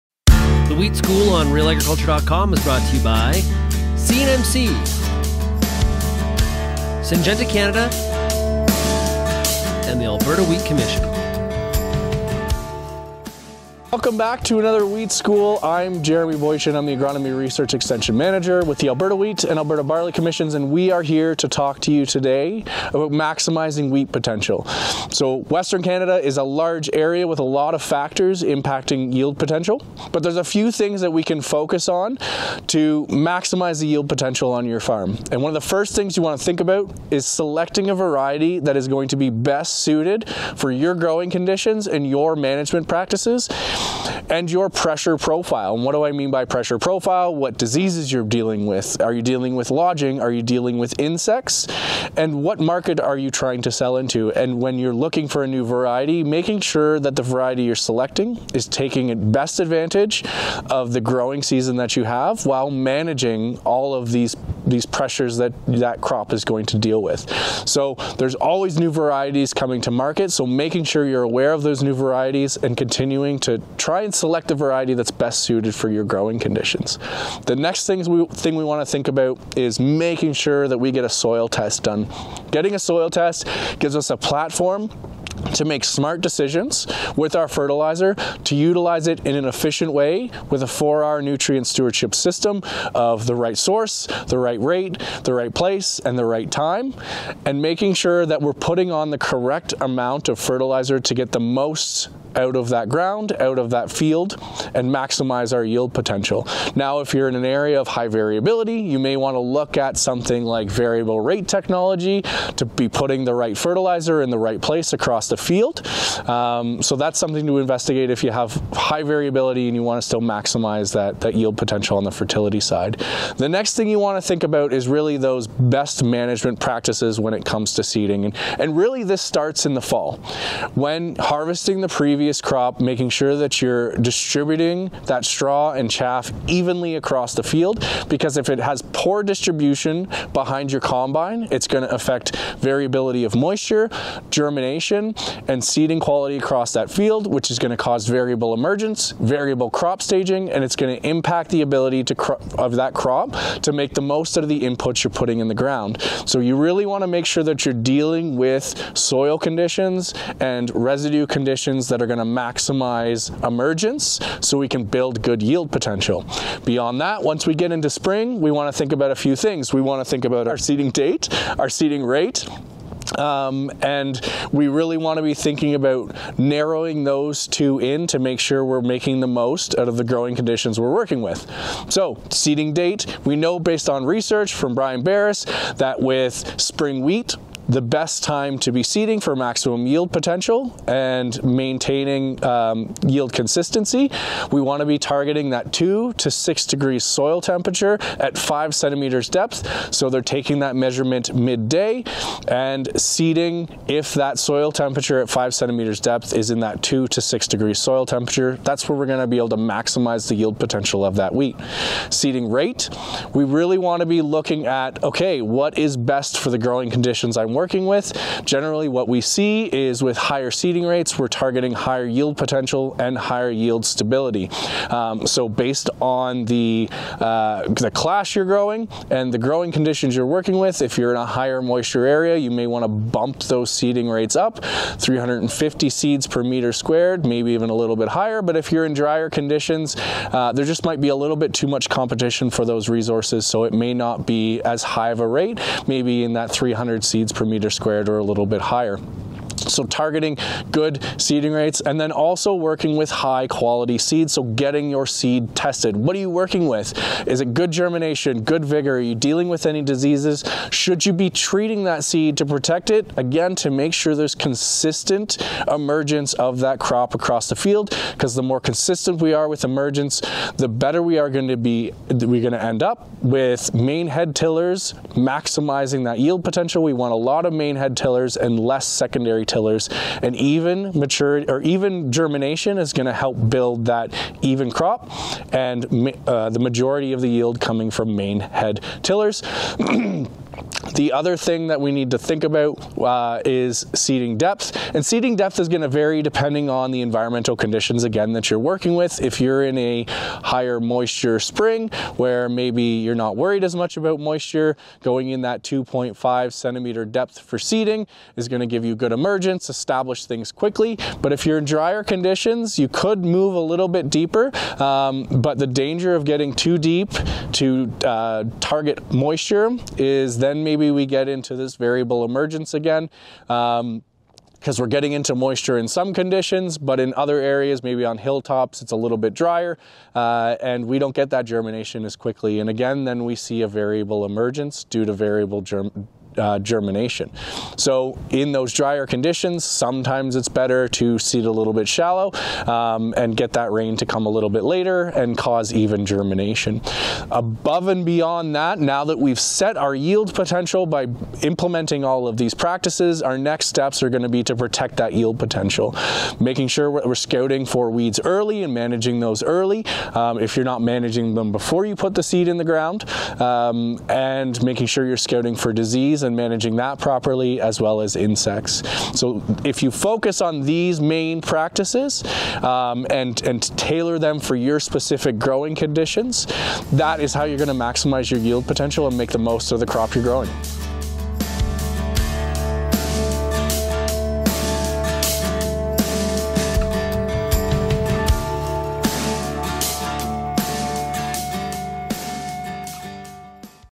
Wheat School: Maximizing wheat performance, right from the get-go
Before  seed even goes in the ground, farmers are asking the question: how do I make the most of this crop? How do I ensure my yield potential is where it should be?

Jeremy Boychyn, agronomy research extension manager with the Alberta Wheat and Barley Commissions, joins us in our latest episode of the Wheat School to discuss what can be focused on across Western Canada in order to maximize wheat performance.

Although Western Canada is a large geography, and we don't want to paint it with one brush, there are a few main things you're going to want to focus on, says Boychyn: selecting the right wheat variety, soil testing, seeding date, and seeding rates and depth.